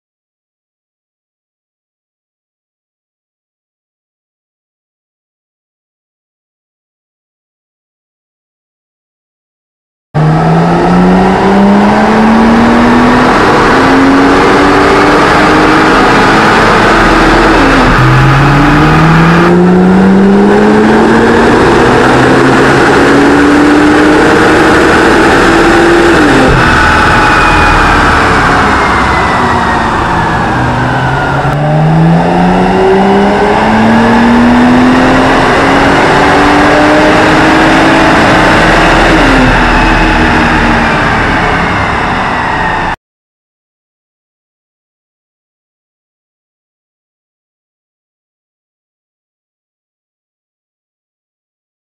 BMW 135i baseline dyno runs before Agency Power Downpipes
Agency Power does some baseline dyno runs on a 2008 BMW 135i AT.

Car has a Burger Motorsports JB4 Tuner and Remus axle back exhaust.

Testing was done on a Mustang Dyno which we all know shows lower numbers.  We're just base-lining the car to show how the Agency Power Downpipes will perform next.